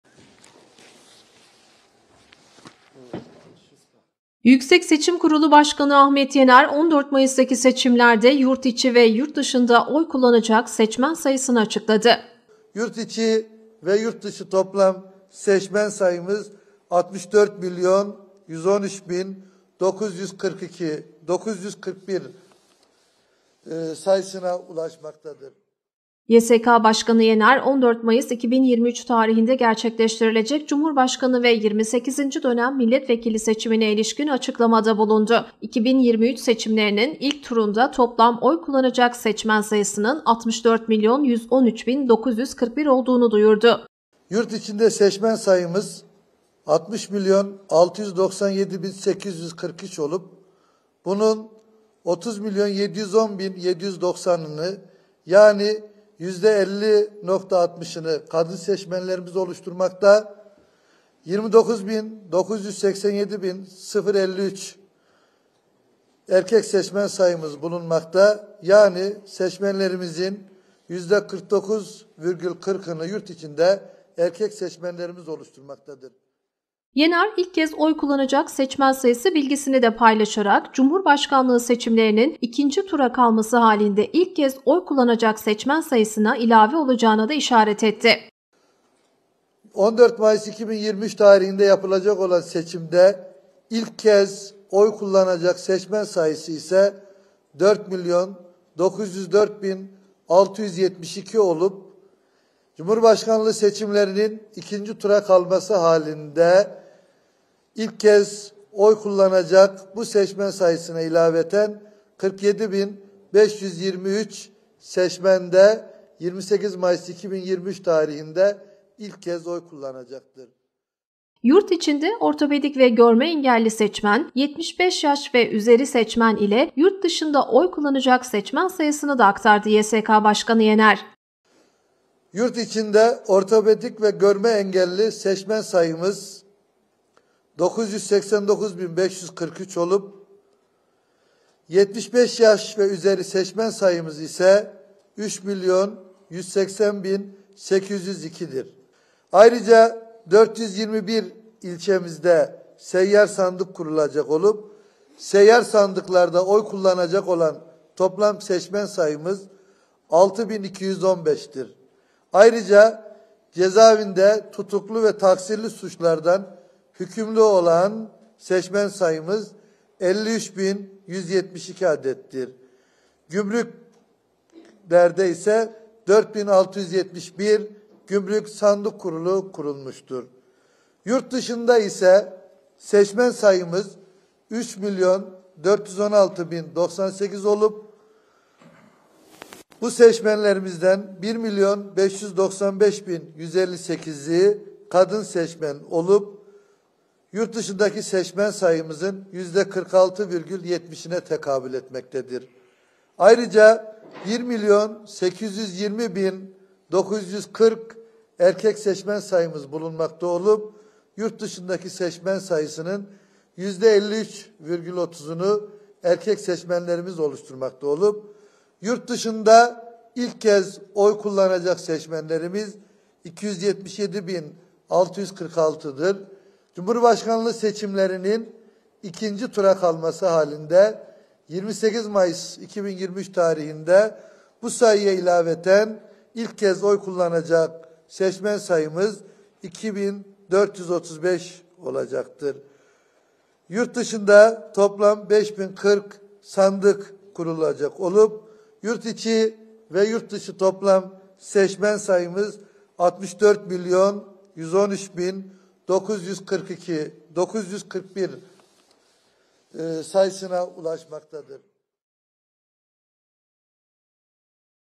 YSK BAŞKANI YENER SEÇMEN SAYISINI AÇIKLADI (17.04.2023)
YSK Başkanı Yener, 14 Mayıs'taki seçimlerde yurt içi ve yurt dışında oy kullanacak seçmen sayısını açıkladı.